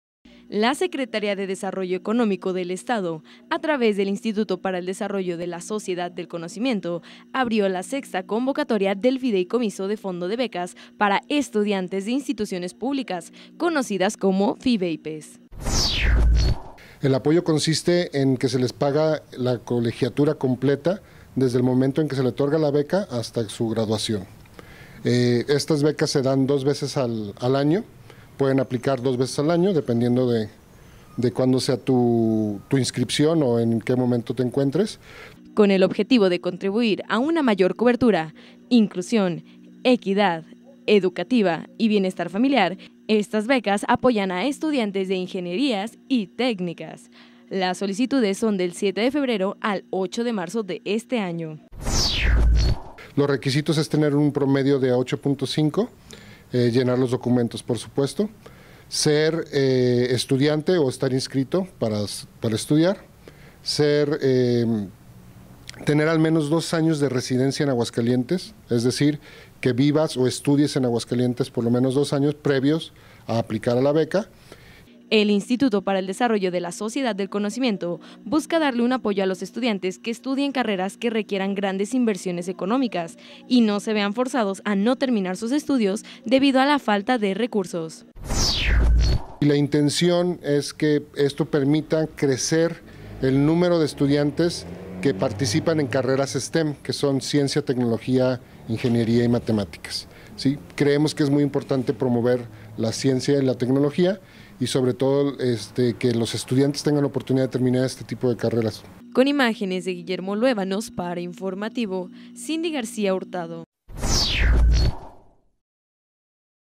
APOYAN A UNIVERSITARIOS PARA QUE SIGAN ESTUDIANDO
La SEDEC y el IDSCEA abren la sexta convocatoria del fideicomiso de fondo de becas para estudiantes de instituciones públicas.